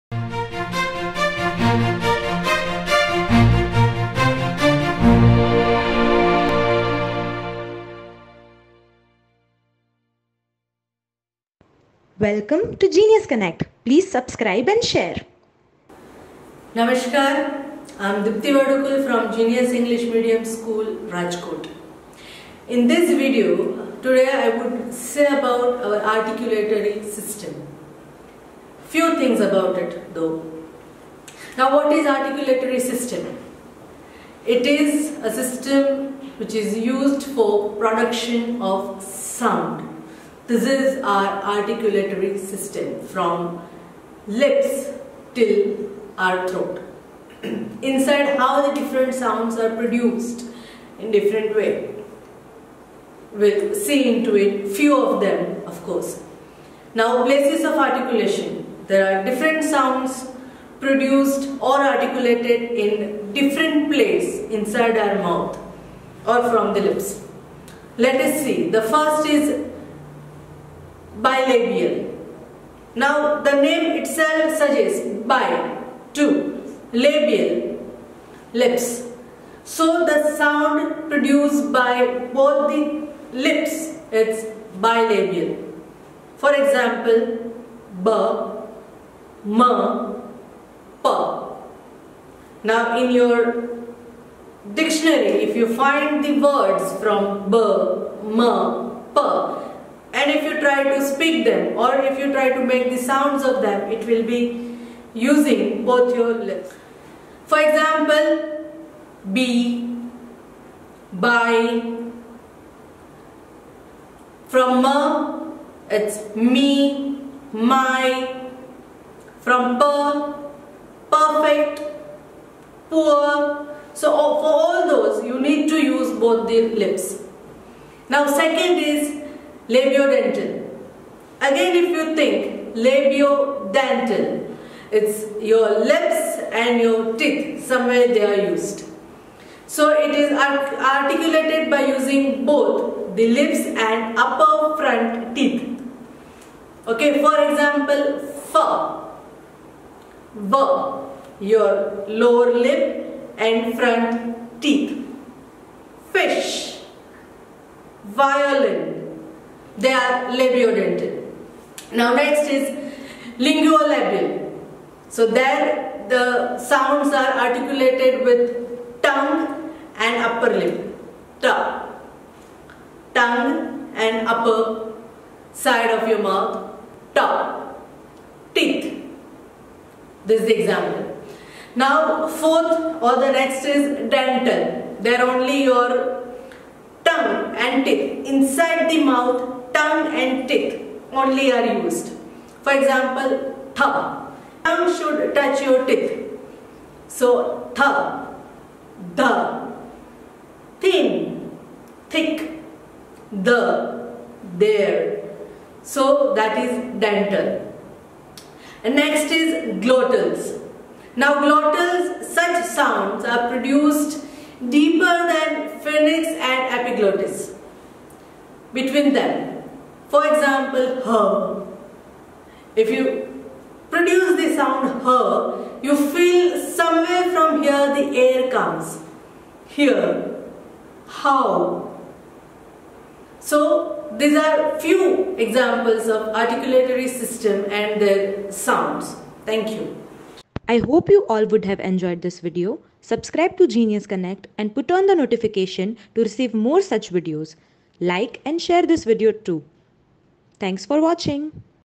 Articulatory System|Sounds|Genius School
English educator at Genius School Ms. Dipti Vadukul explained the Articulatory System with examples.